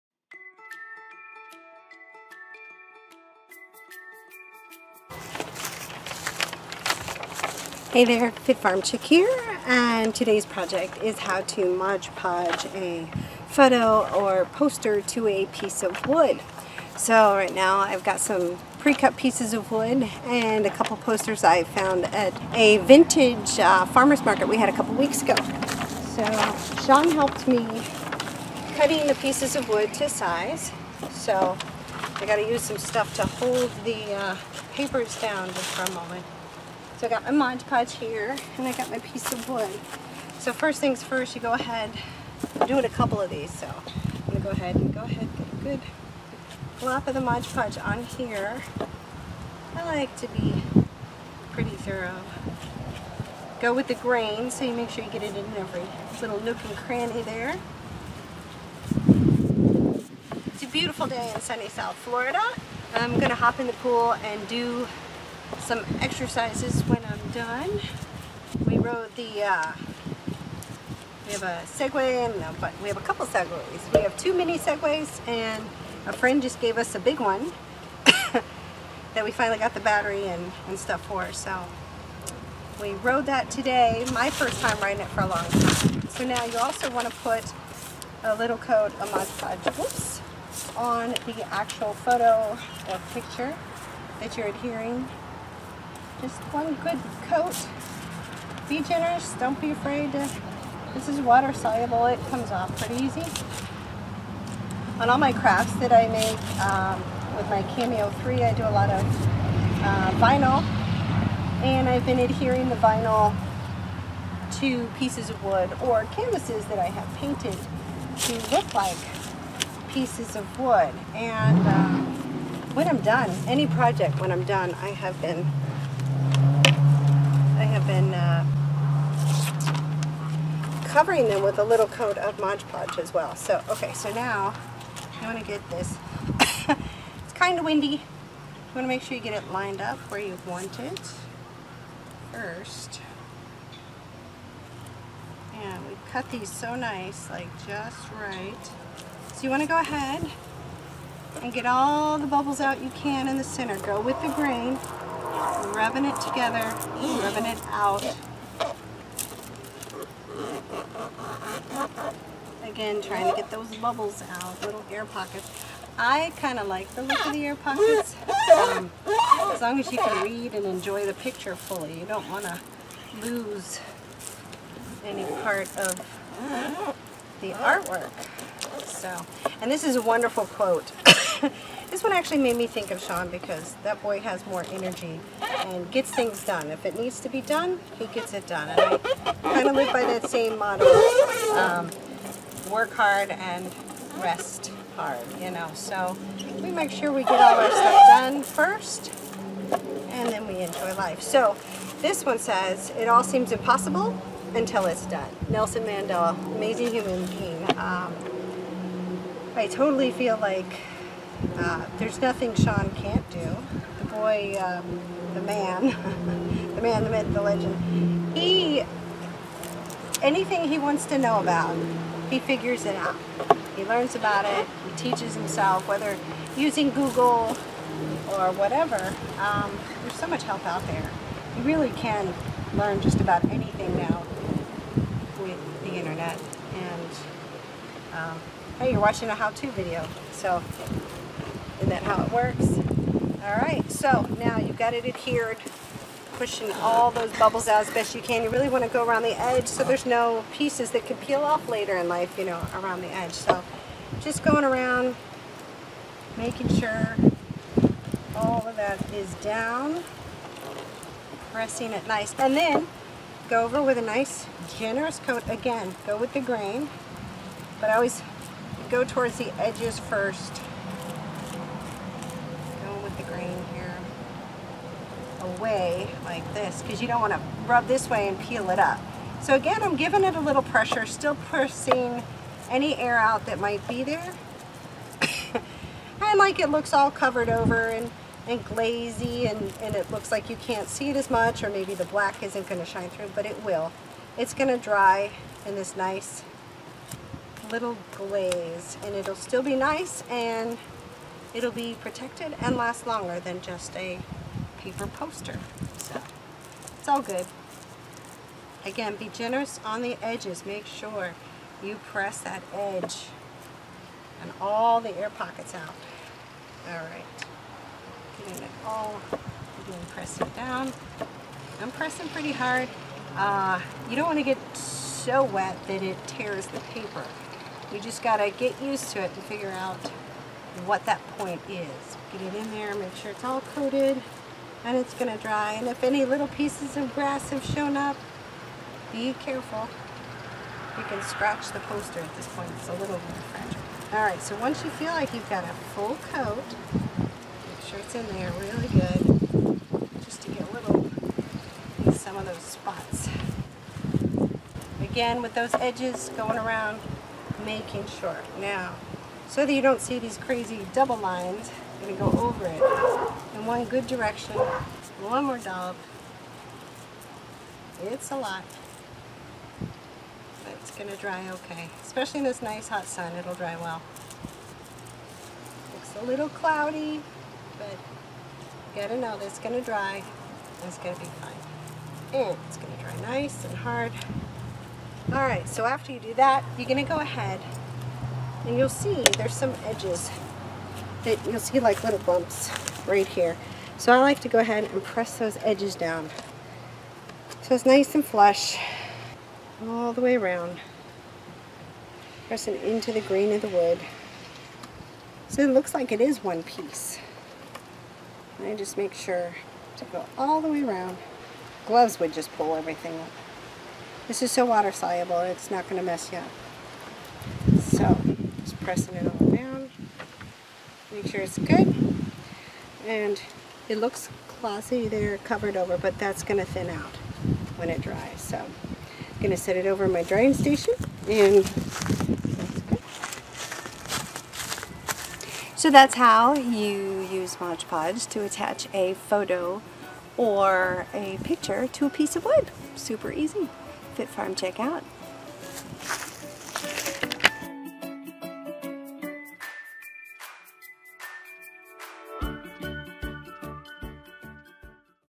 Posters or photos on wood using Mod Podge
How to adhere any poster or photo using Mod Podge